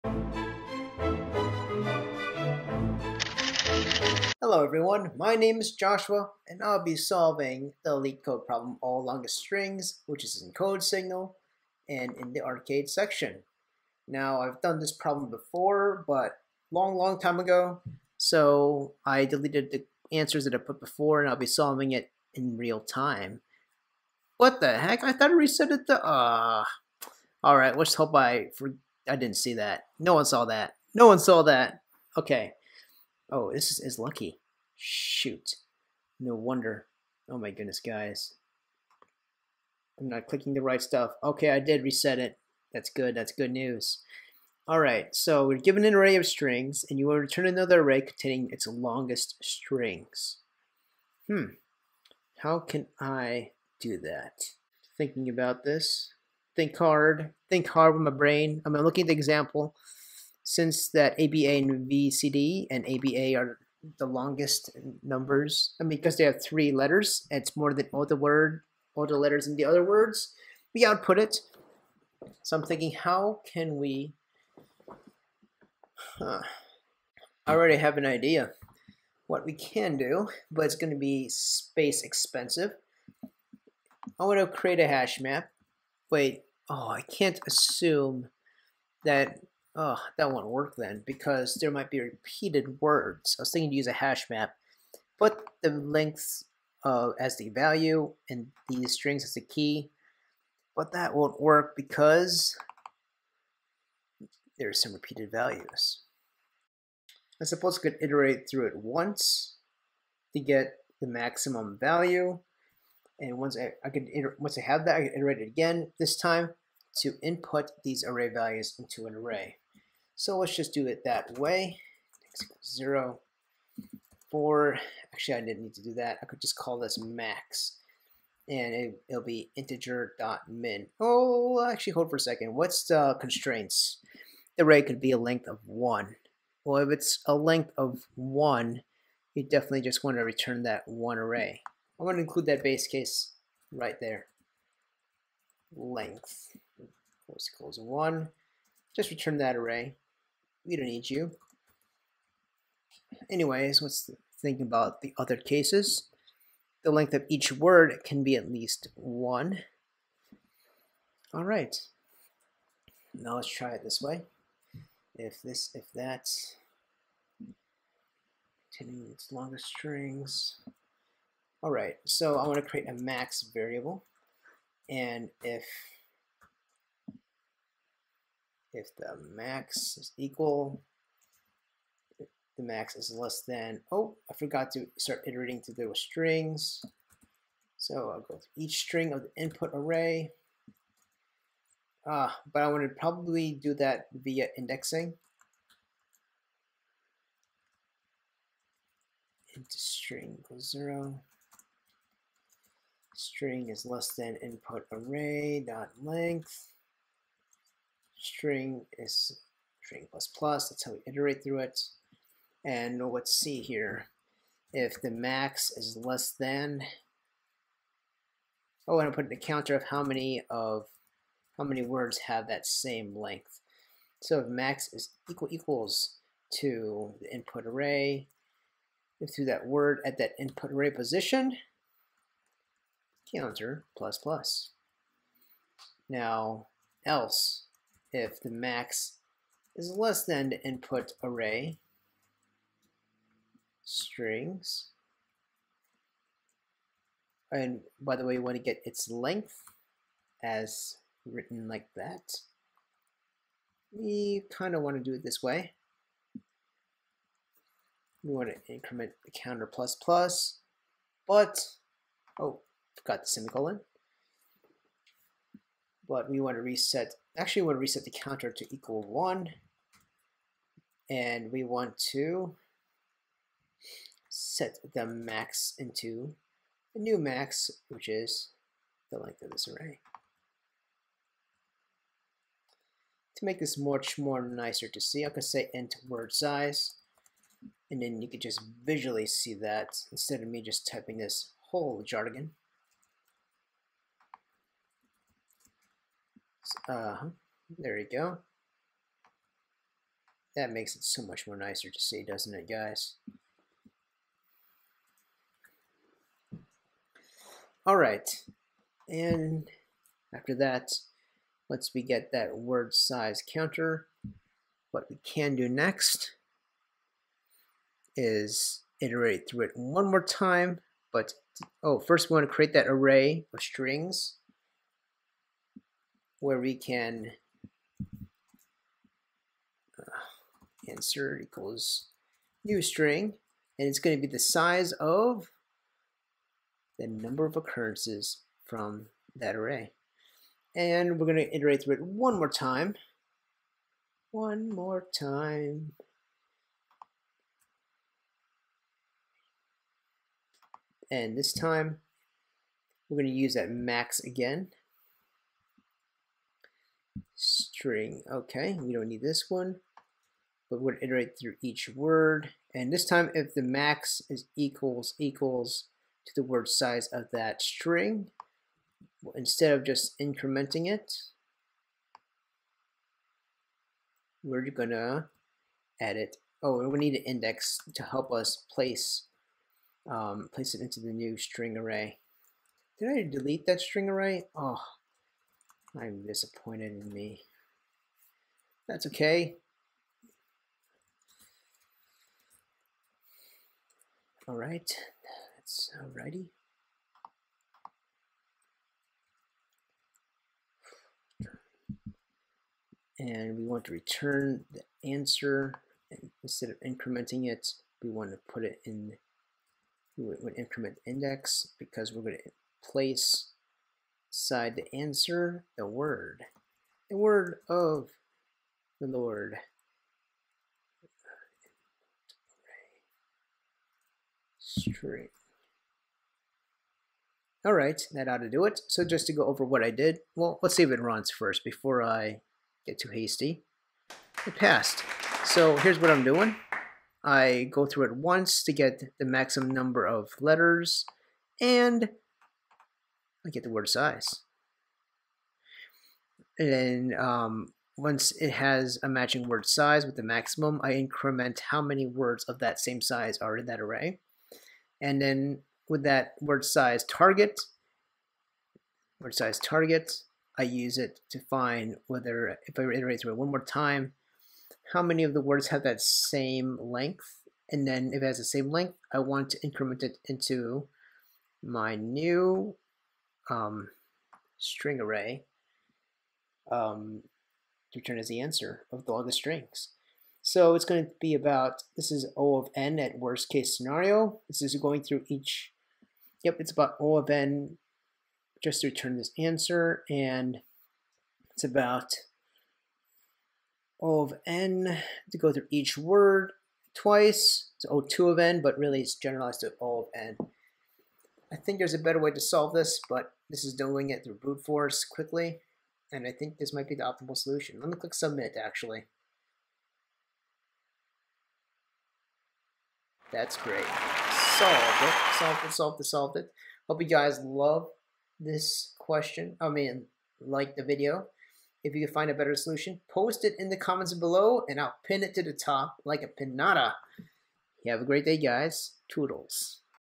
"All Longest Strings" - Codesignal #9 - JAVA Solution
Or give advice or alternative solutions in the comments below!

Given an array of strings, return another array containing all of its longest strings.